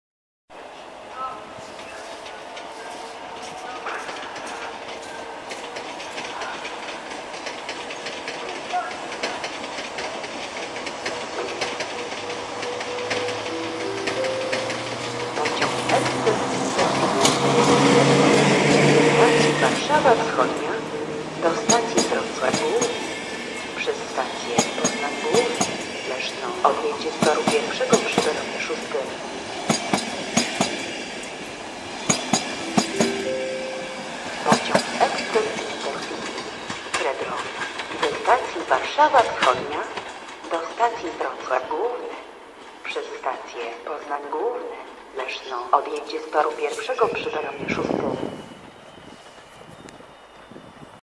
EP09-010 z EIC 1603 Fredro odjeżdża ze stacji Warszawa Zachodnia.
EP09-010 z EIC 1603 Fredro relacji: Warszawa Wschodnia - Wrocław Główny odjeżdża ze stacji Warszawa Zachodnia.
Data nagrywania filmu: 2 kwietnia 2013.